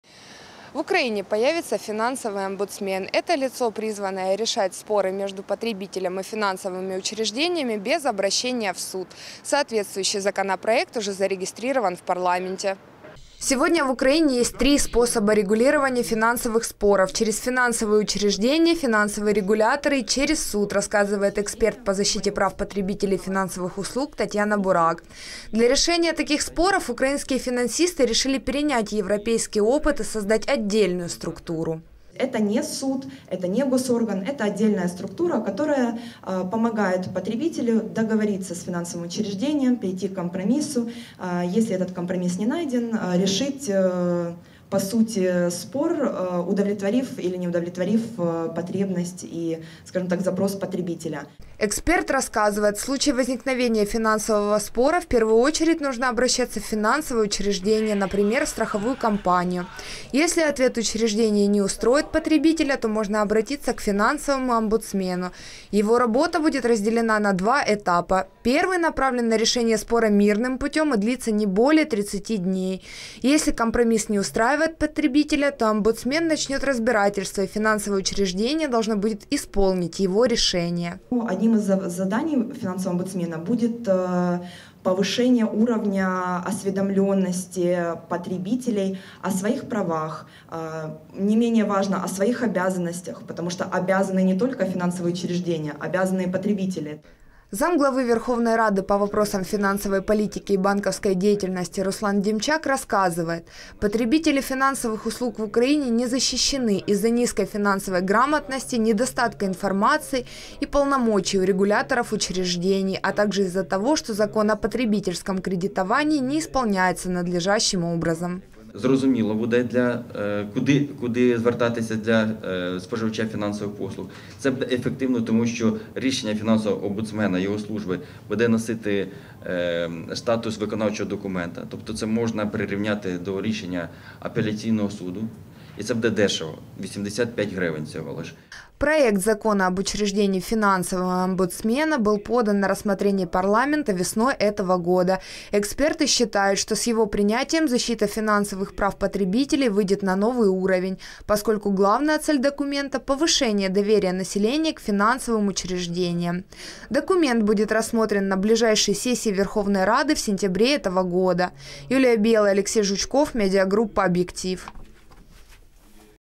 В Украине появится финансовый омбудсмен
Его задачей станет решать споры между потребителями и финансовыми учреждениями без обращения в суд. Сегодня в Украине есть 3 способа регулирования финансовых споров: через финансовые учреждения, финансовые регуляторы и через суд. Для решения таких споров украинские финансисты решили перенять европейский опыт и создать отдельную структуру.